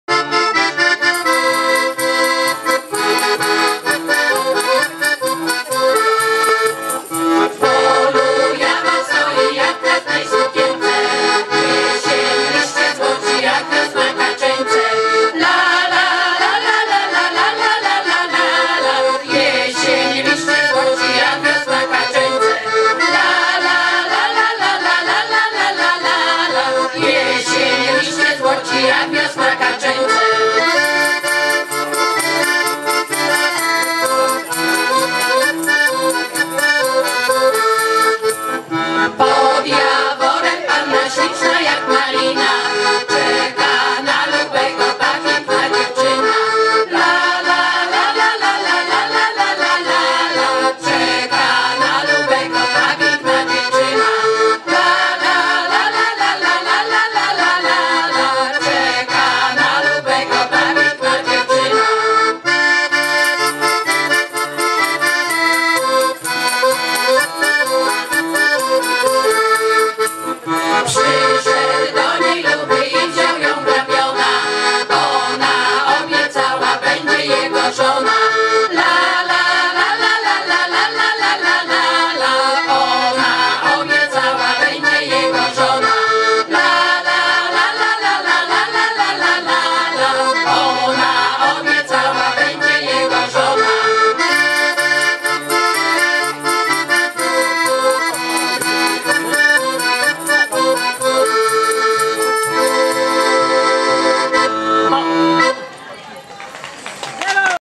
Zespół "Stypułowianie"
XI Przegląd Zespołów Śpiewaczych. Stypułów, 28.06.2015 r.